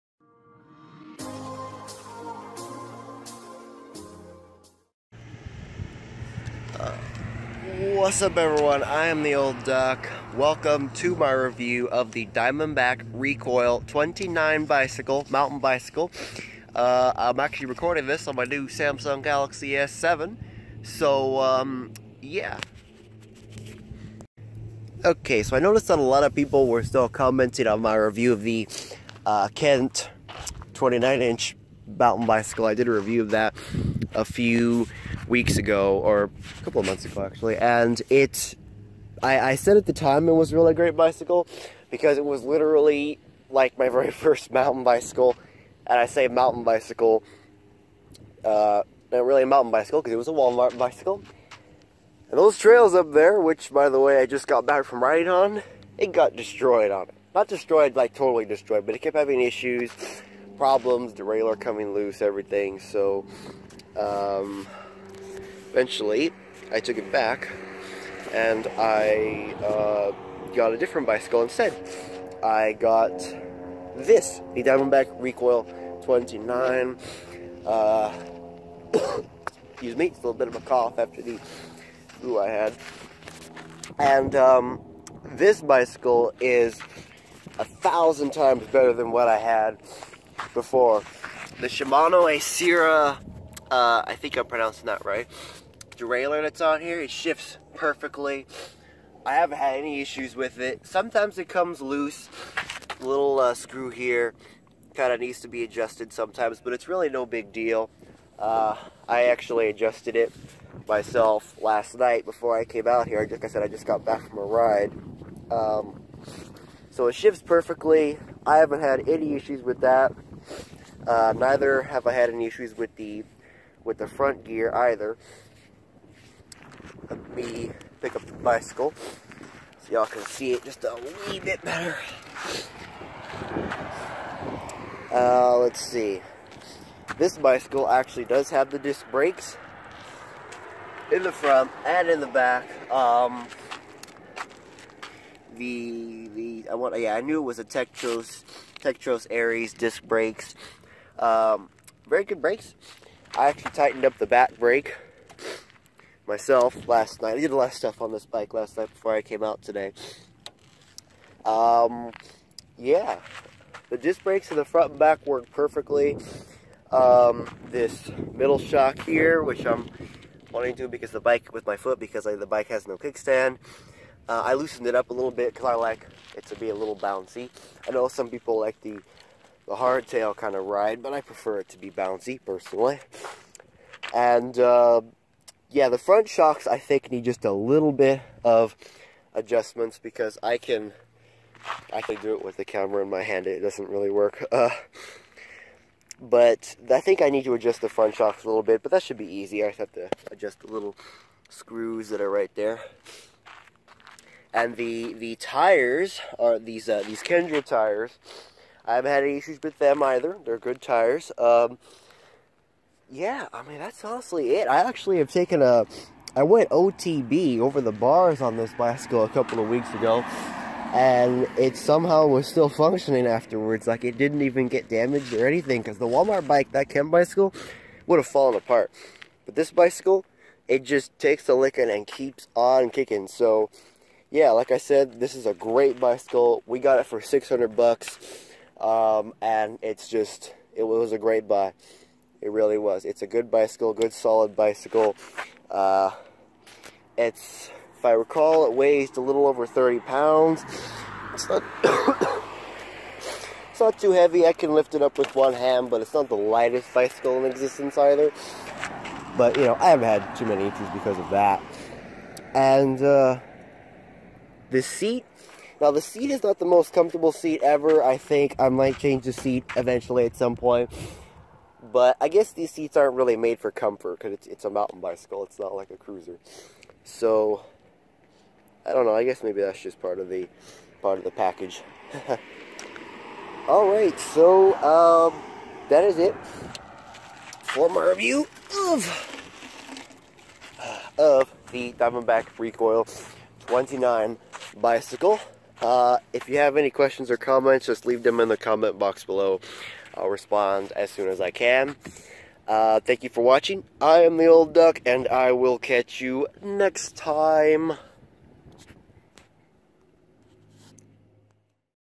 2017 Diamondback Recoil 29" Mountain Bike Review
A quick review/overview of the 2017 Diamond Recoil 29er.